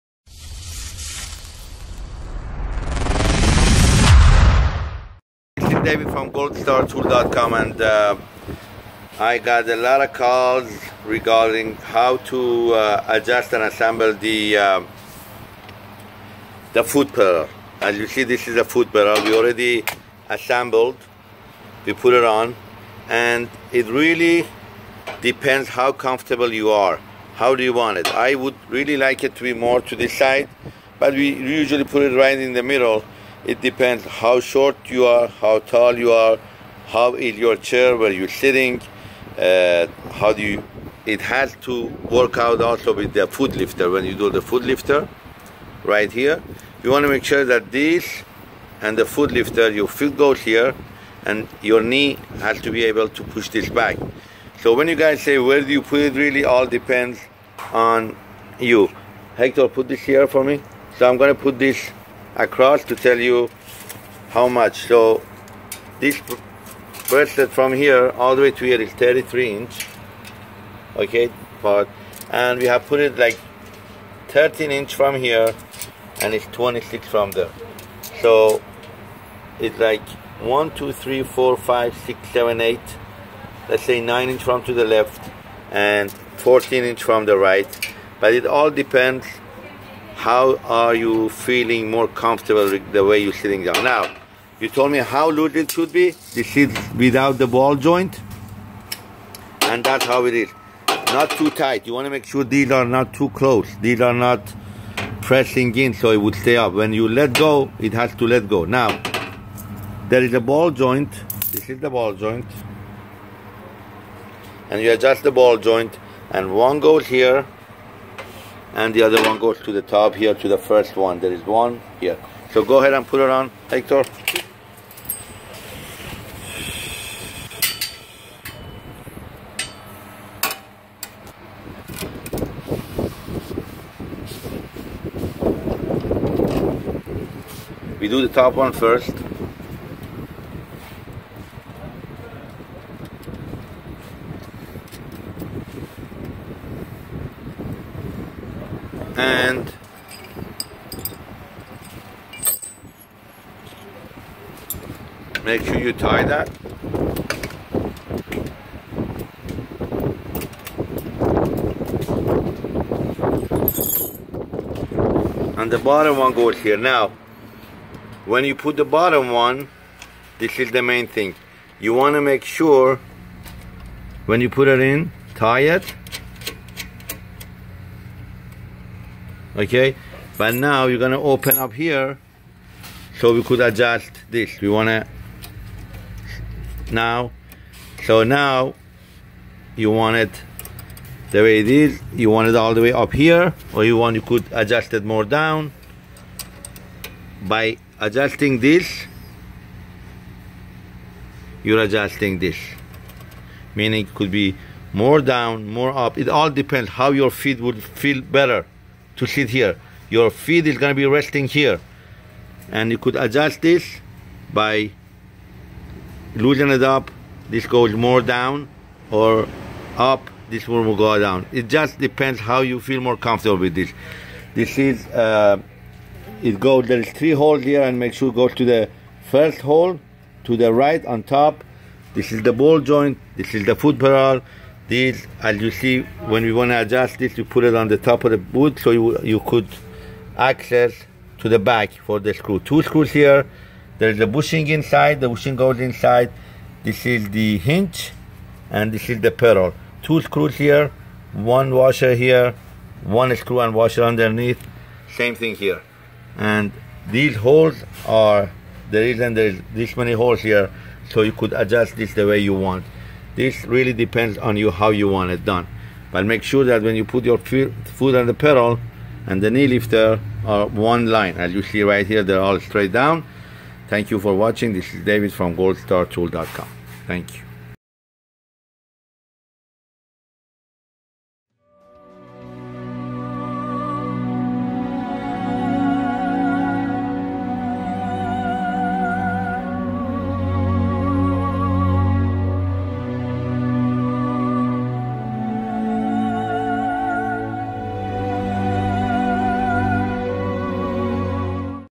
Tutorial - Adjusting & Assembling Juki DDL-8700 Foot Pedals - Goldstartool.com
Today David demonstrates how to adjust and assemble pedal & ball joints for Juki DDL-8700 and all other industrial sewing machines!

Juki DDL-8700 High-Speed Single Needle Straight Lockstitch Industrial Sewing Machine w/Table and Servo Motor